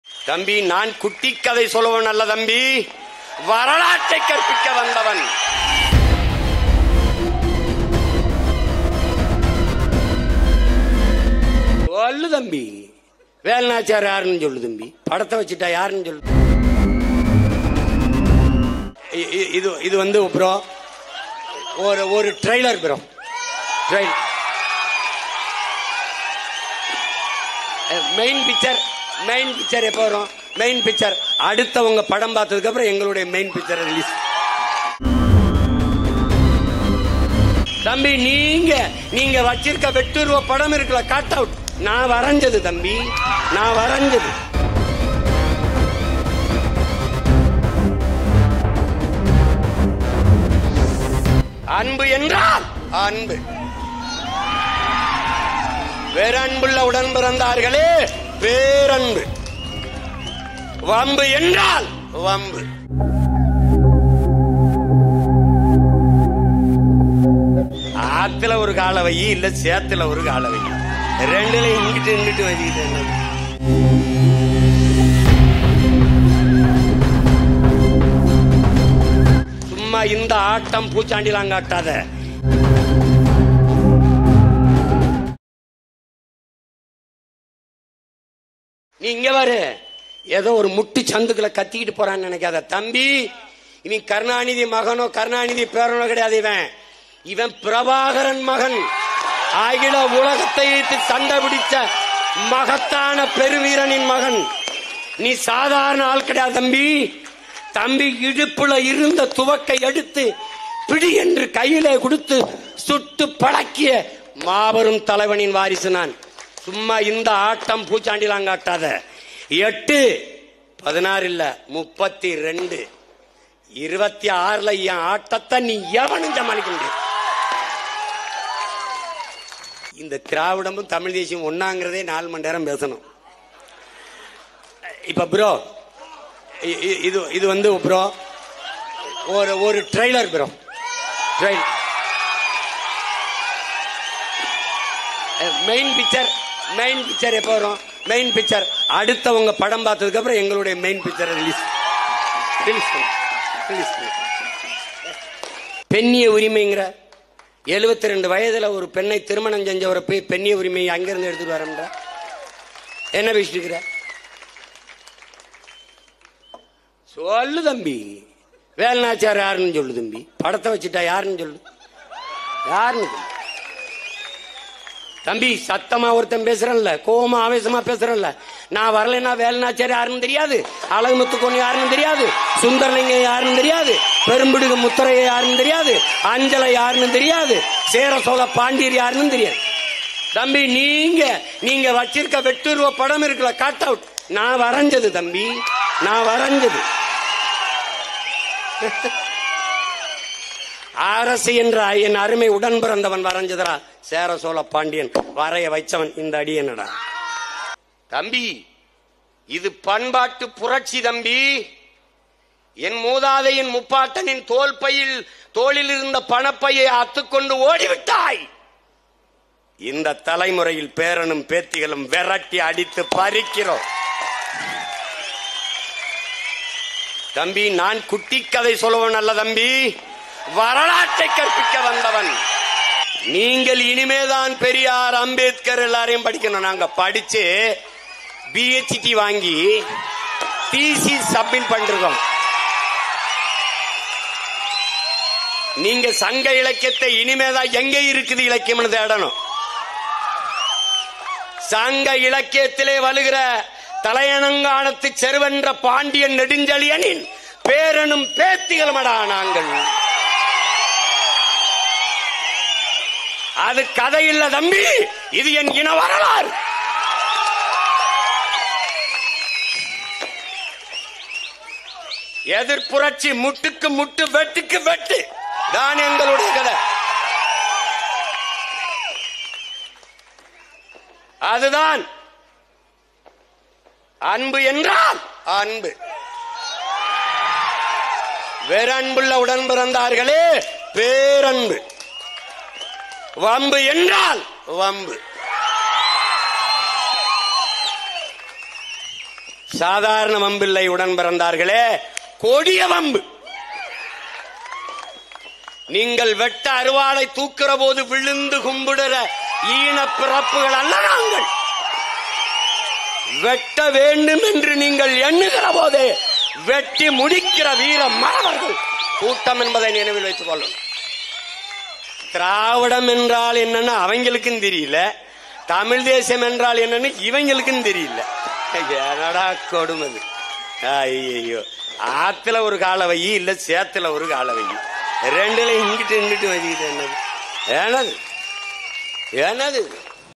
"சும்மா இந்த ஆட்டம் பூச்சாண்டி எல்லாம் காட்டாத!.." TVK தலைவரை தாக்கி பேசிய சீமான்! | vijay | seeman
"சும்மா இந்த ஆட்டம் பூச்சாண்டி எல்லாம் காட்டாத!.." TVK தலைவரை தாக்கி பேசிய சீமான்! "குட்டிக்கதை சொல்பவன் நான் அல்ல தம்பி.."